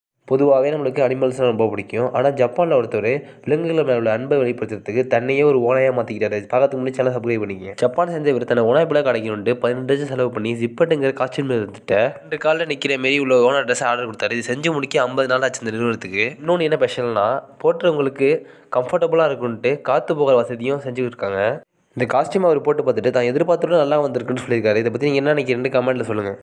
Human transformation to dangerous wolf 😱😱
ஓநாய் போல் மரிய வினோத மனிதன் 😱😱

Man in Japan spend 18 lakh transform into wolf